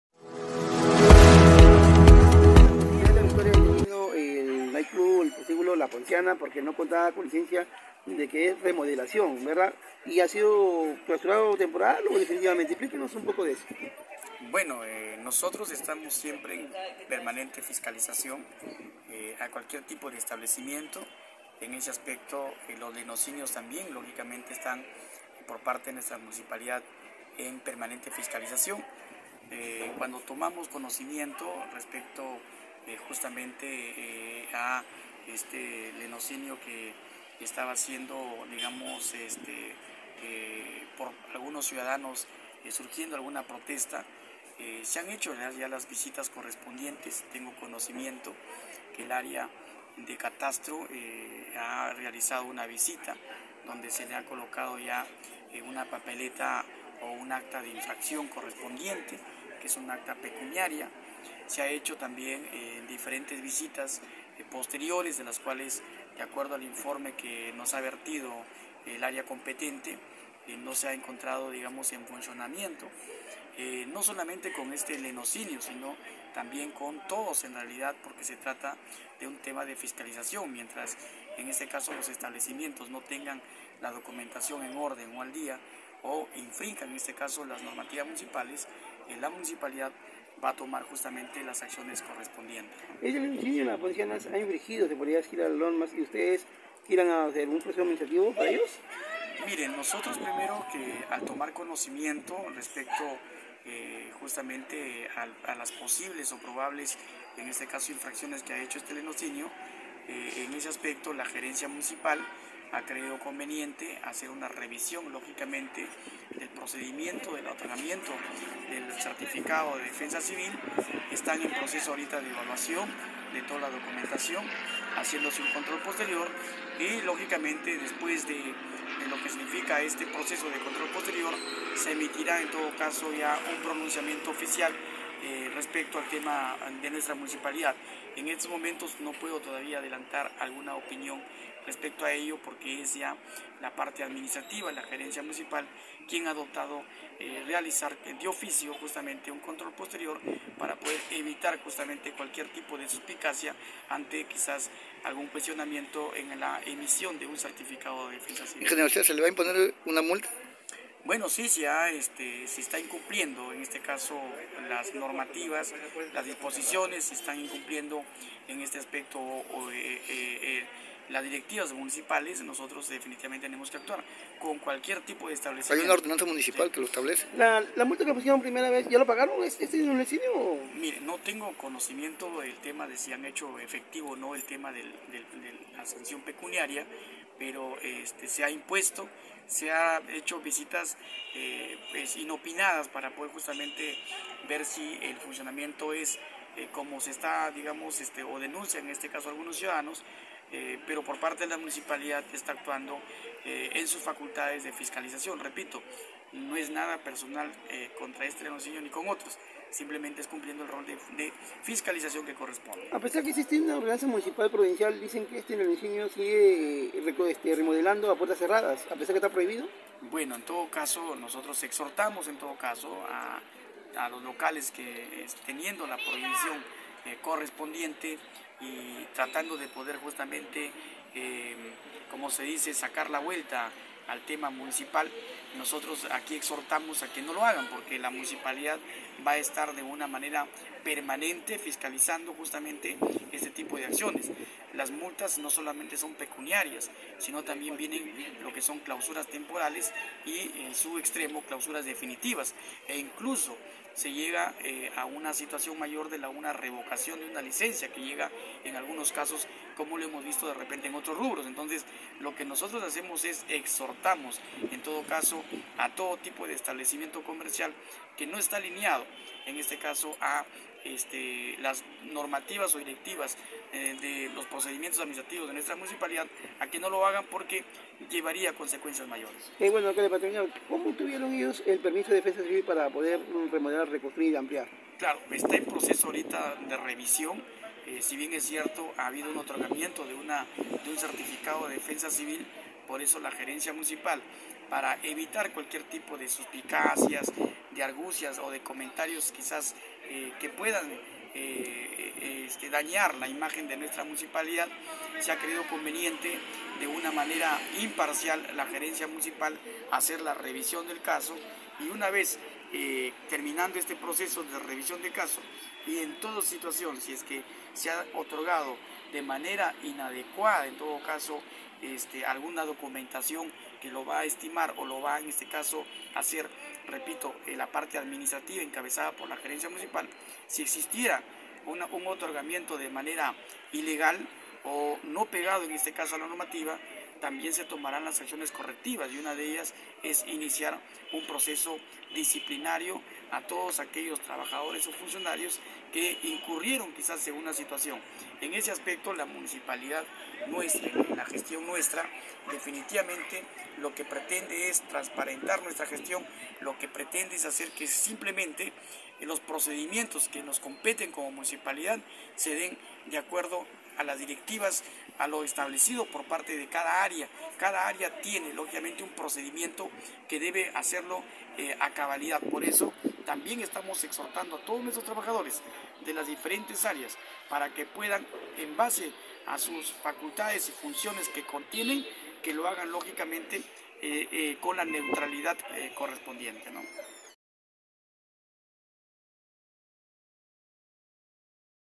La Poncianas no podrá reaperturar por haber remodelado sin contar con el permiso correspondiente
El Alcalde de La Victoria Ing Edwin Vasquez Sánchez, señalo que vienen realizando constante supervisión a cualquier tipo de establecimiento y de los Leneocinios igual.

La Municipalidad Distrital de La Victoria viene fiscalizando para que no continúen con los trabajos de remodelación. Además le abrirán un proceso administrativo por desacato y resistencia a la autoridad.